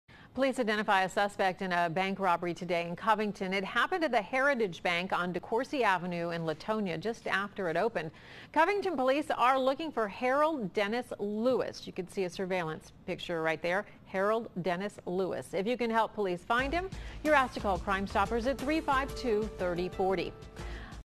Covington bank robbed; suspect identified
COVINGTON, Ky. (WKRC) -- Covington police are looking for a bank robber.The thief, identified as Harold Dennis Lewis, targeted the Heritage Bank at 36th and Decoursey Avenue at about 9:15 a.m. Friday, May 1. Police have not released any other information about the crime.